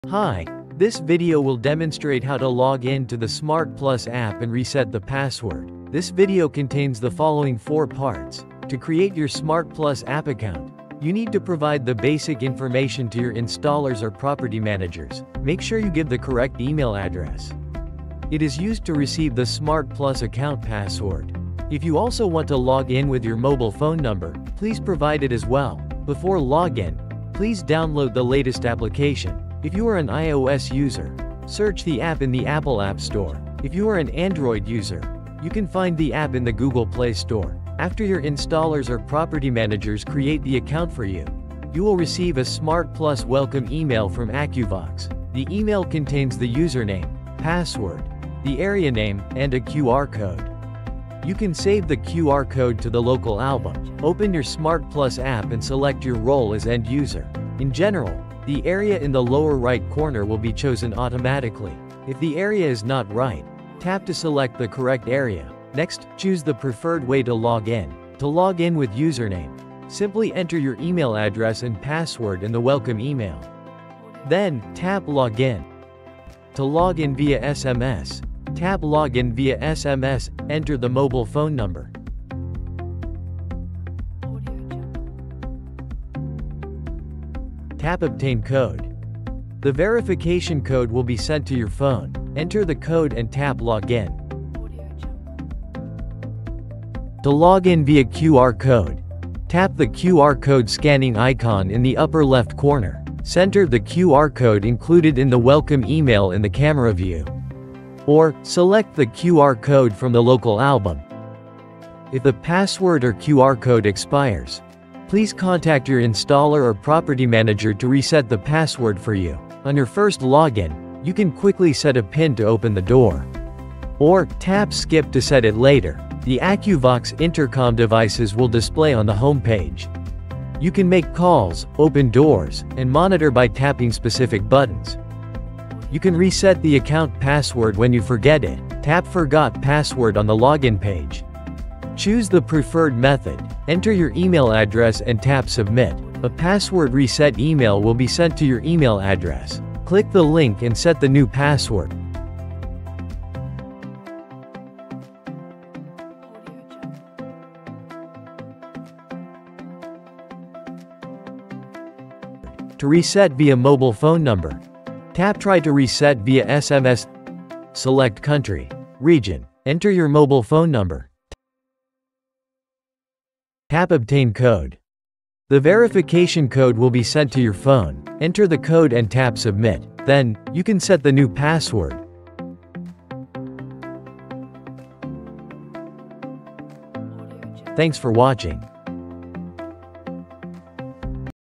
SmartPlus App Login and Password Reset
00:00 Overview
00:10 Preparation
00:55 Log in to the SmartPlus App with Different Methods
02:15 Reset the SmartPlus App Password